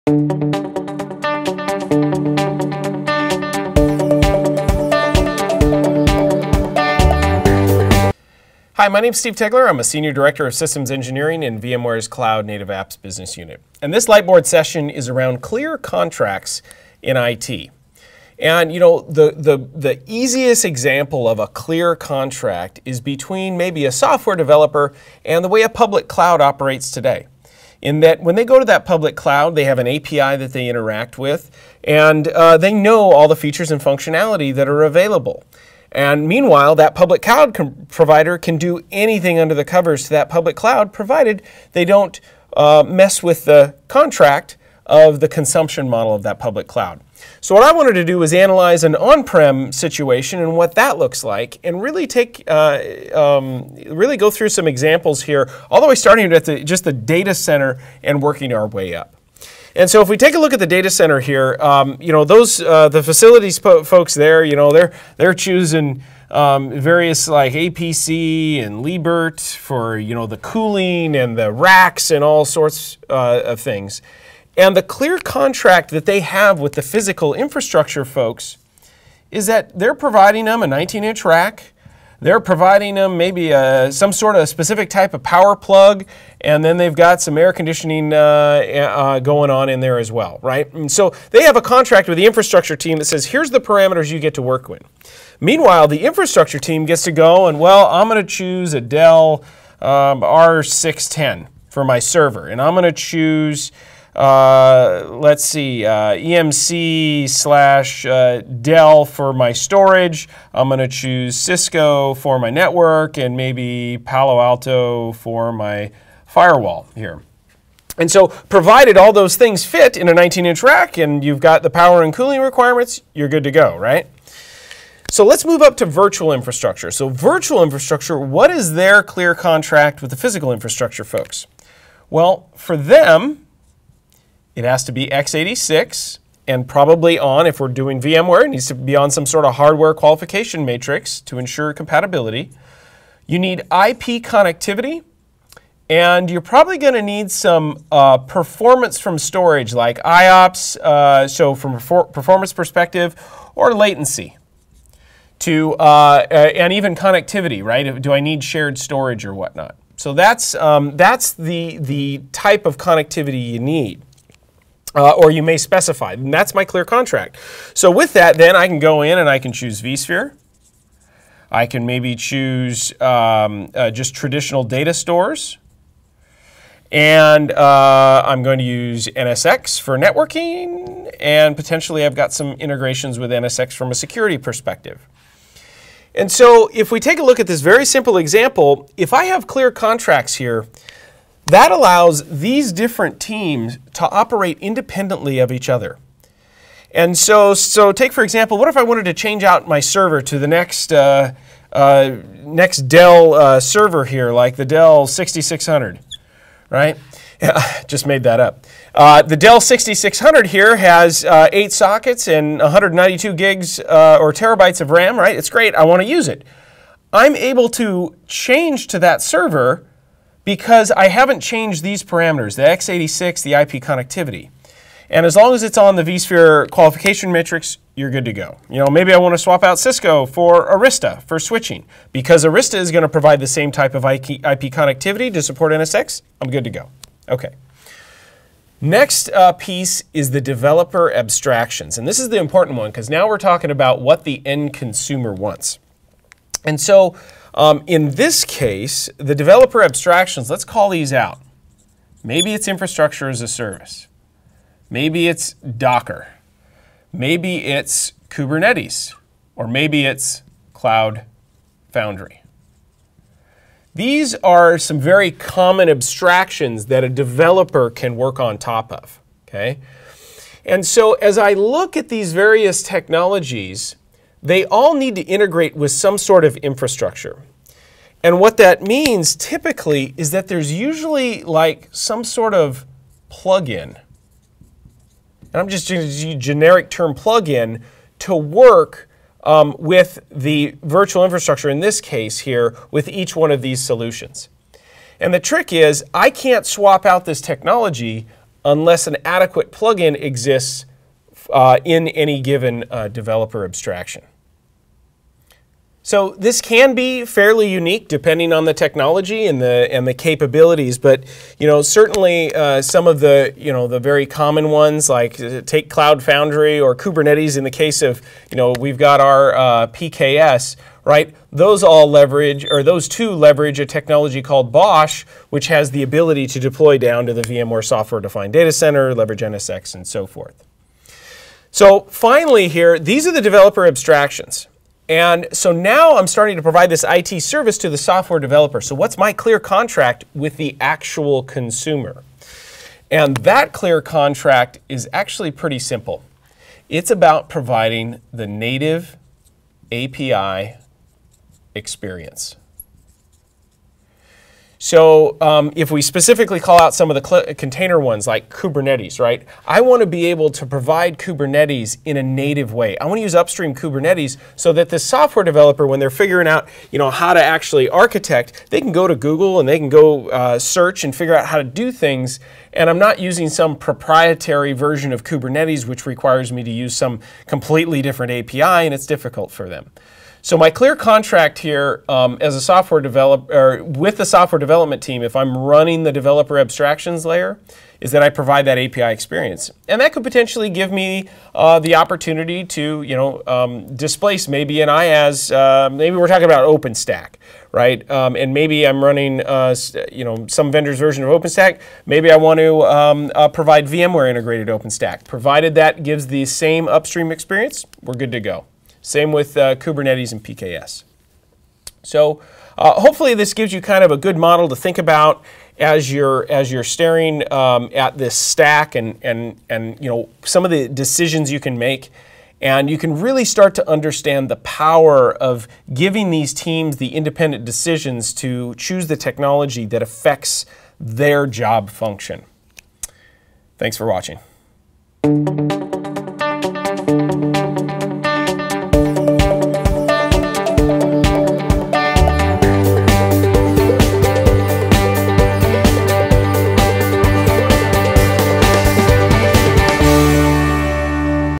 Delivering IT Service through Clear Contracts Between IT Abstraction Layers
In this lightboard session, Steve Tegeler analyzes the layers in an IT organization in the on-prem model starting at the datacenter and working up to developer abstractions, and explains the clear contracts between each of the layers.